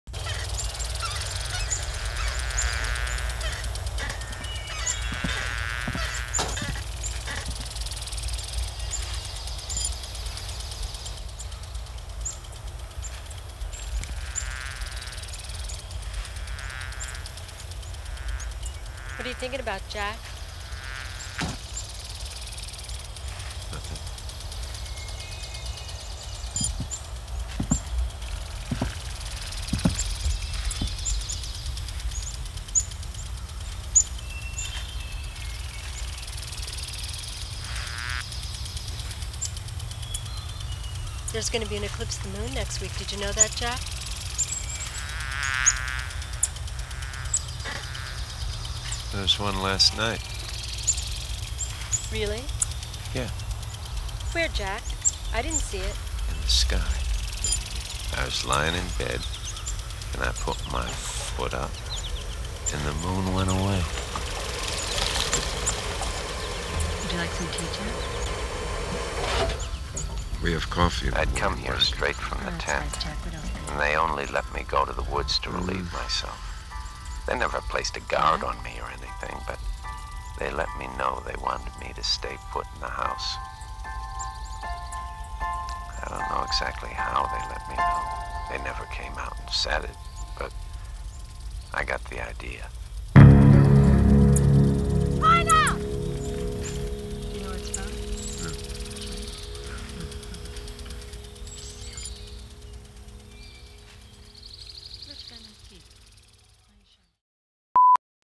from the archives an excerpt from WRONG GUYS Marilyn's cabin 1997
In this video: Willem Dafoe and Kate Valk